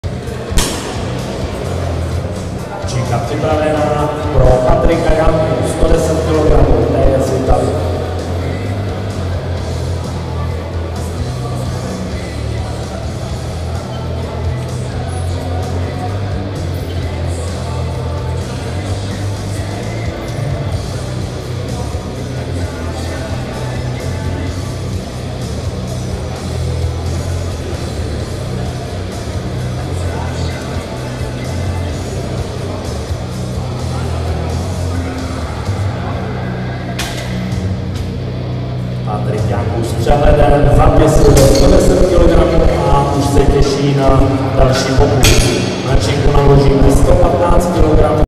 Janků Patrik (1998), BP1, 110 kg
12. Mistrovství ČR v benčpresu dorostu, juniorů a masters

Místo: Sportovní hala v areálu plaveckého bazénu, Jihlava

Federace: ČSST
Pořadatel: Powerlifting Jihlava